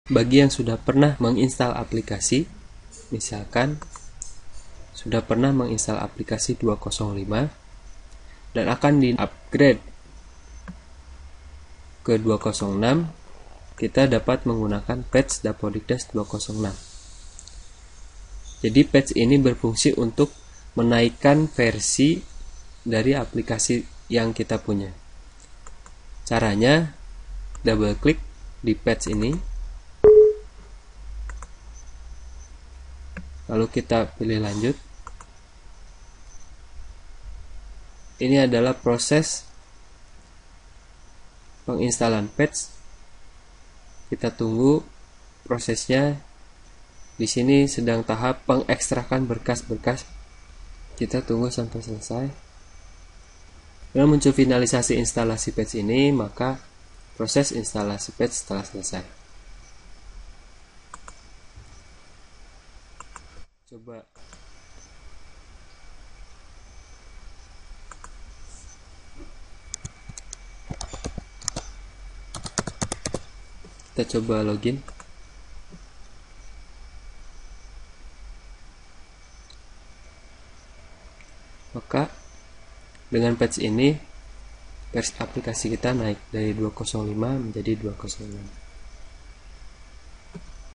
Tutorial lengkap install patch di dapodikdas
Mengulas lebih lengkap tentang bagaimana caranya menginstal patch dapodikdas dengan mudah dan lancar